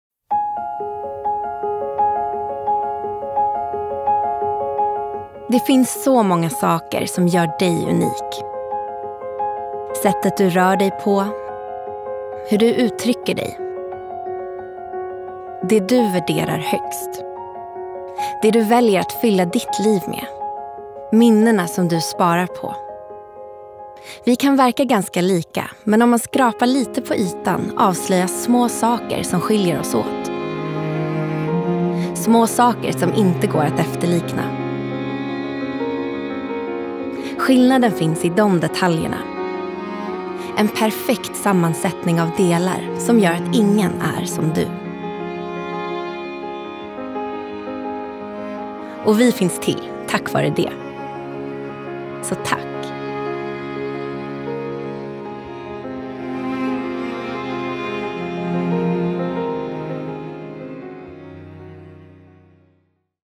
Ingen är som du!
Ingen är som du. Det är tack vare det, som Fingerprints finns, och därför våra produkter fungerar. Så vi vill passa på att hylla det unika i varje människa. Tack!